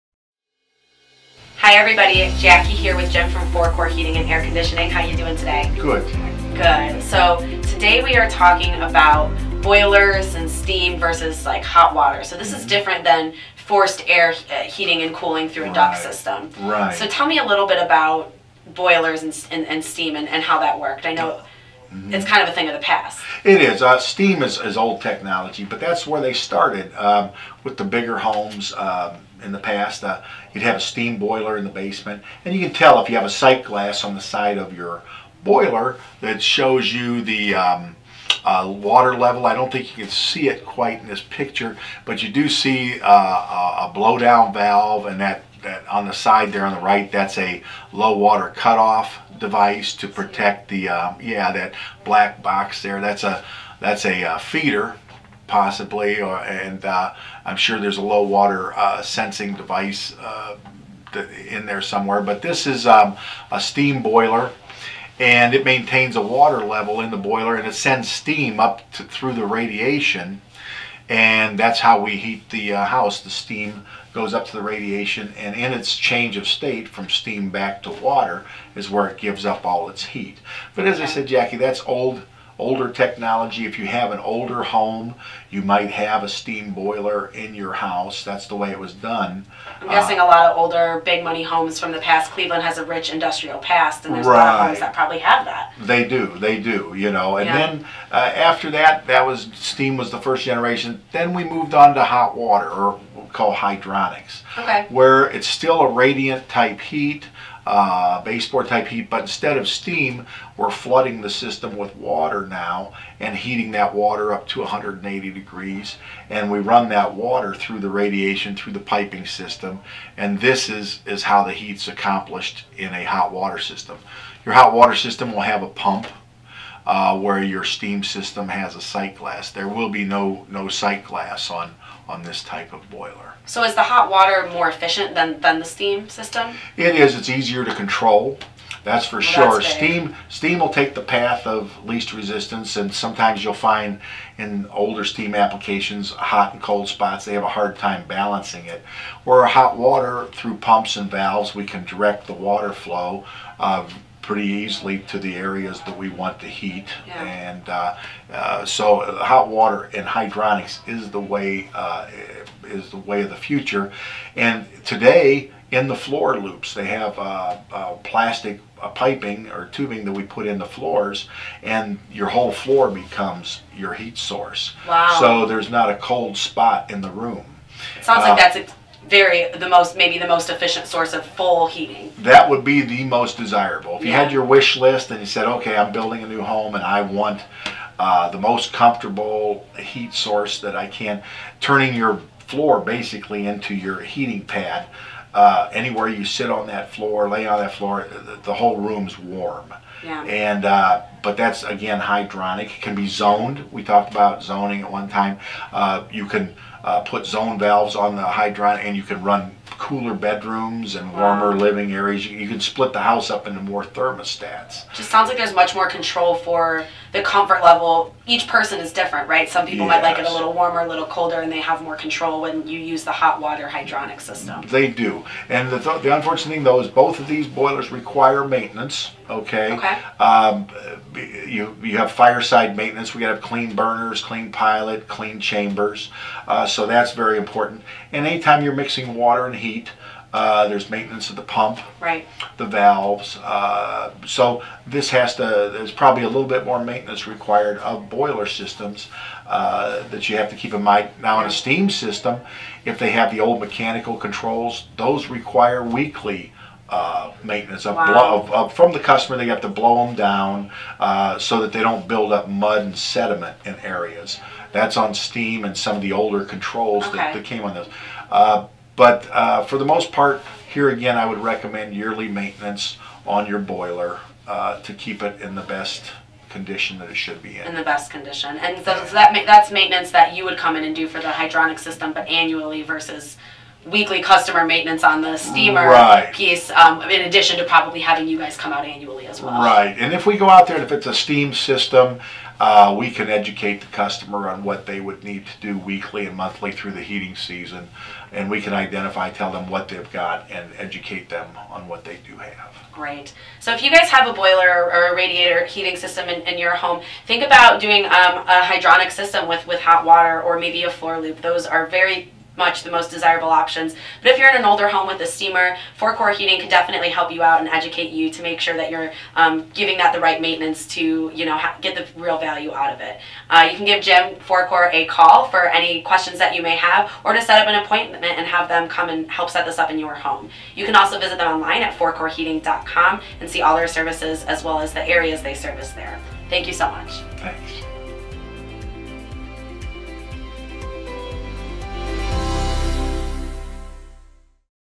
Maintenance Requirements for Steam vs. Hot Water Boilers
If you have a boiler or radiator heating system in your home think about doing a hydronics system with hot water or a floor loop. Those are very desirable options. If you're in an older home with a steamer, Forquer Heating can help you out and educate you to make sure you're giving that the right maintenance and getting the most value out of it.